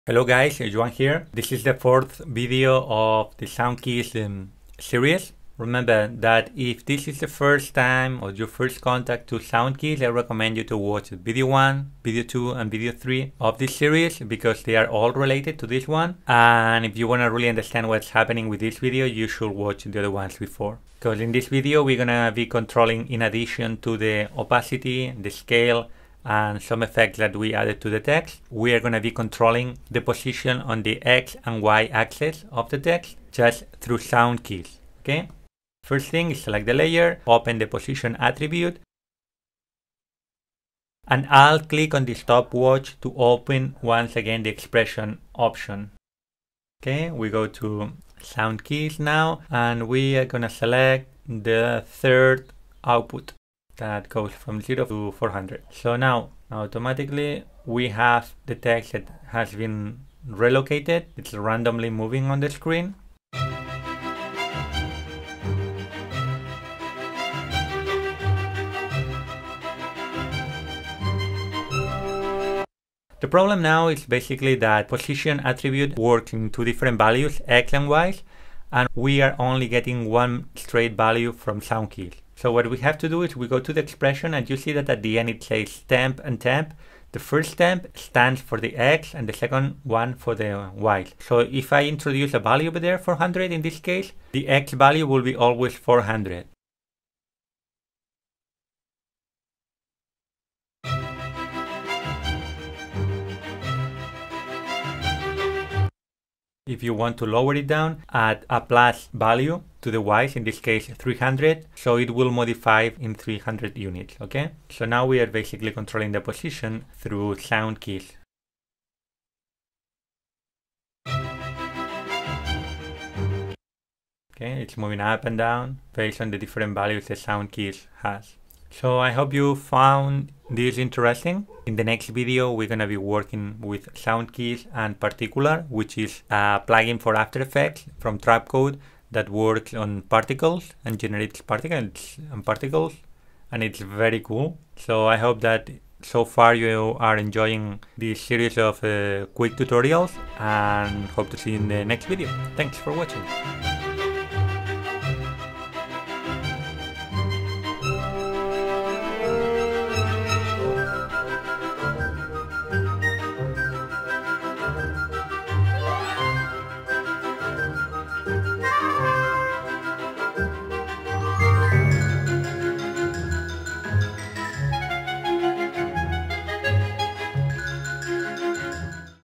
Sound Keys Lesson 4 - Tutorial to Trapcode plugin for AE
Sound Keys is an awesome plugin from Trapcode for Adobe After Effects. In Lesson 1 we will learn how to control a layer's position on the composition through Sound keys, in addition to the opacity, scale and some effects, based on the beats of a sound file.